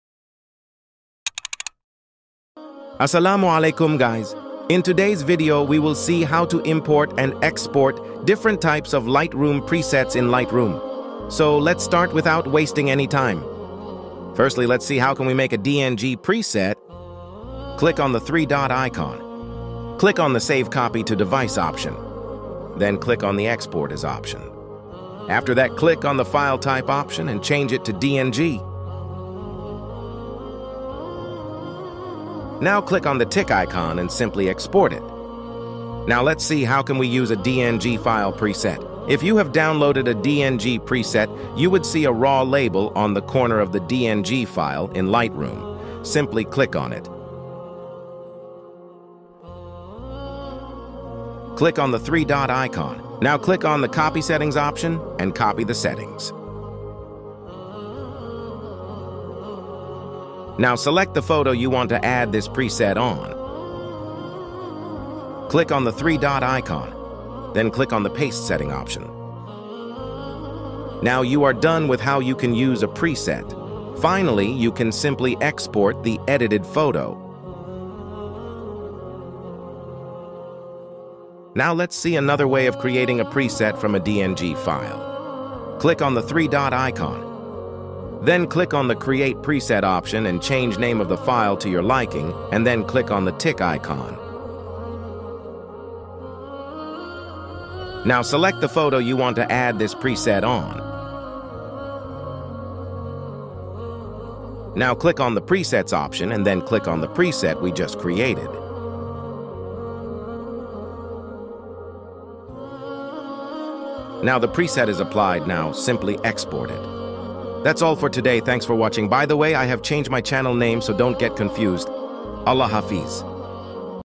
How To Share Lightroom Mobile Presets (Updated 2024)
If you want to share a Lightroom mobile preset with your friends, it seems nearly impossible without the proper steps. Luckily, you can export and share all your favorite presets in seconds once you get the hang of the export as DNG option and sharing to a cloud storage option. I share my presets via Dropbox since that's the cloud storage program that I use, however Google Drive or iCloud will work well for this too. To share multiple lightroom mobile presets at once, you just need to rinse and repeat this process, exporting every preset as a DNG for your friend to save on their phone. Now everyone can get in on the Lightroom preset goodness!
Find More Tutorials Here:

Come Say Hello:
Time Stamps:
0:00 Intro
0:14 How To Create A DNG Preset
0:35 How To Import & Use A DNG File
1:23 How To Save A Preset In Lightroom
1:37 How To Use A Saved Preset
1:57 Outro